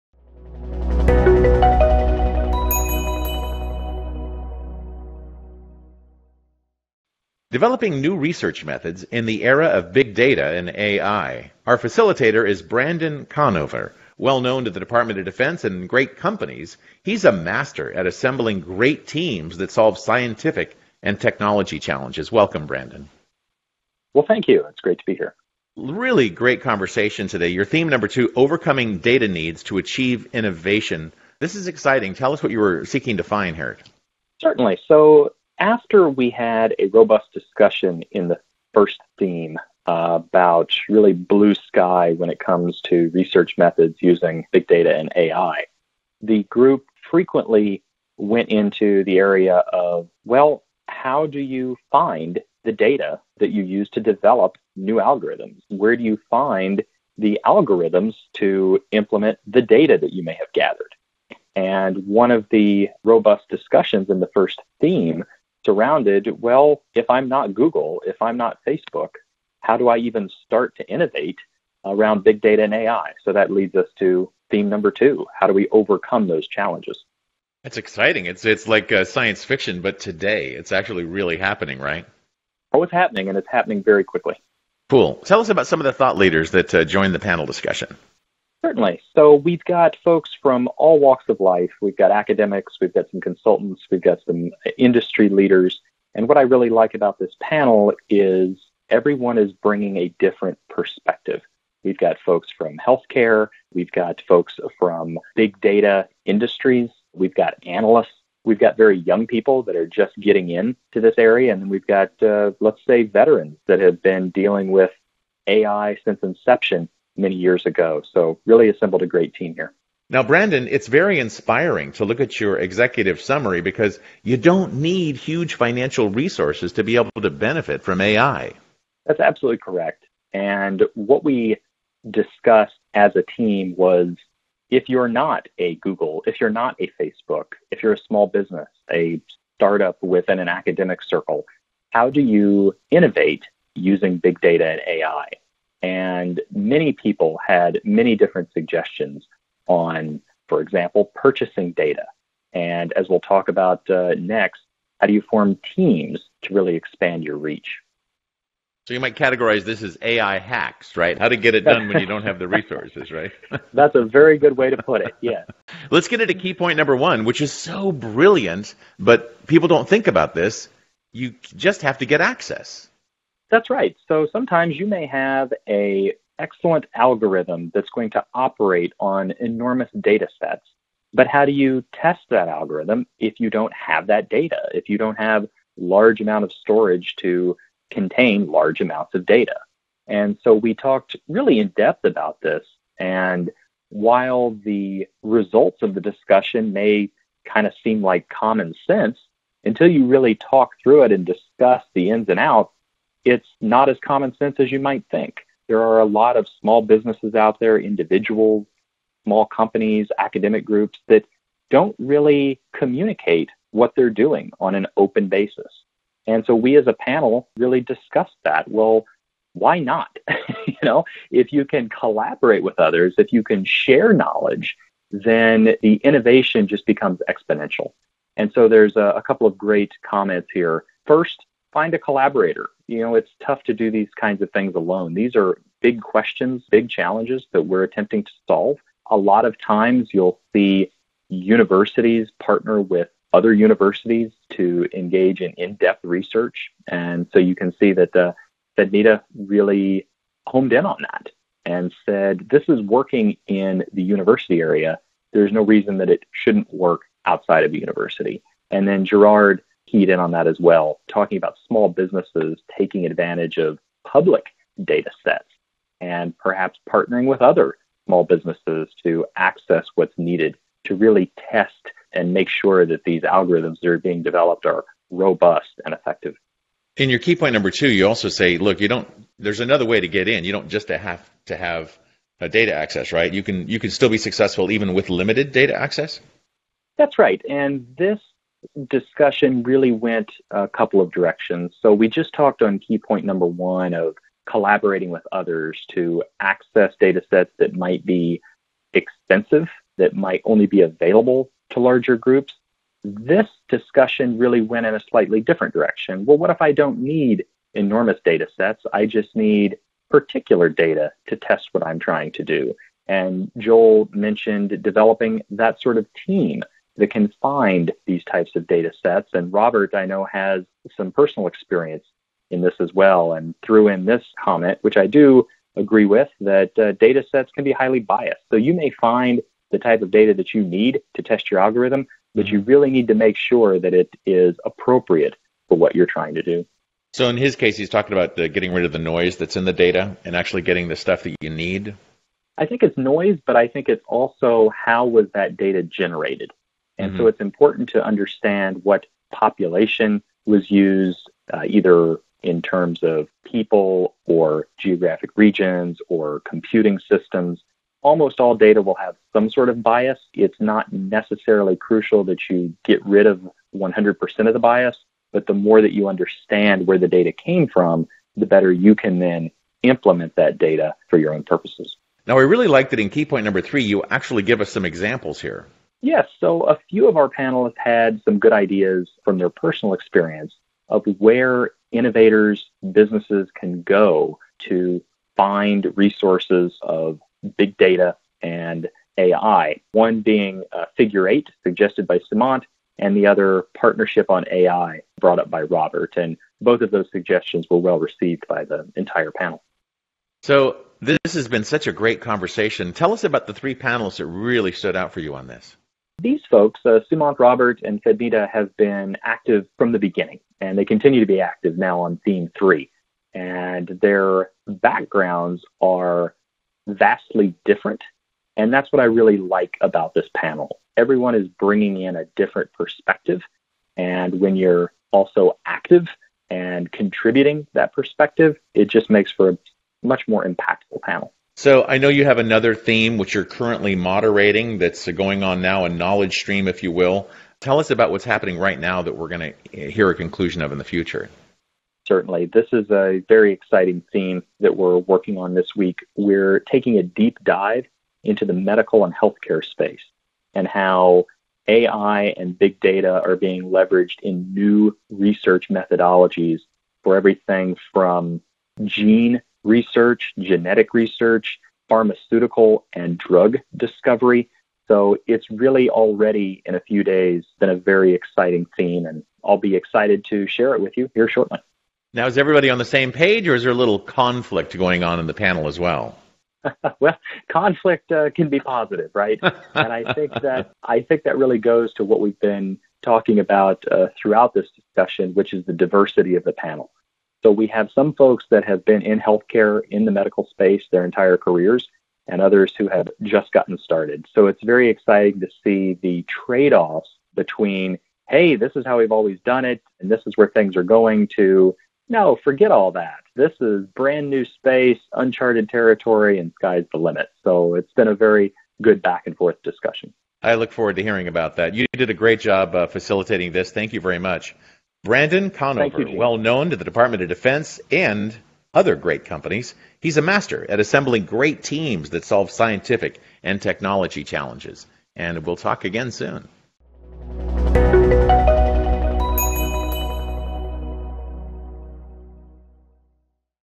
Currnt - Developing New Research Methods in the Era of Big Data and AI - Theme 2 - Recap
Here is a high level summary of the findings:
Small businesses and academic groups are finding success in the development of research using Big Data and AI despite challenges in data access and computing requirements.

 - Innovation doesn’t require deep pockets – especially in this still-early phase of development. Major costs can be overcome by collaboration (e.g., pooling resources and talent).
 - As companies expand in the age of Big Data, more services will become available, including Data As A Service (DAaS), allowing research teams to access information needed to solve critical problems.